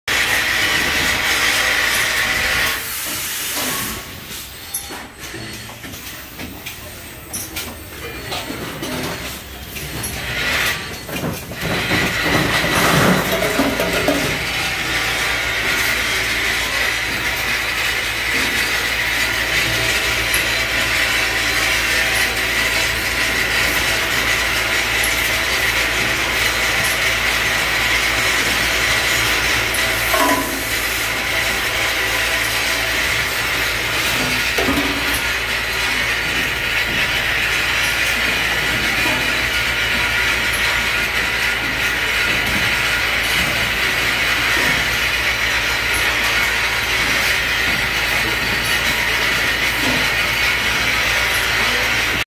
Automatic assembly of fittings on pipes with robots
Automatic assembly of fittings on pipes with 3 robots with fitting orientation check and pipe orientation check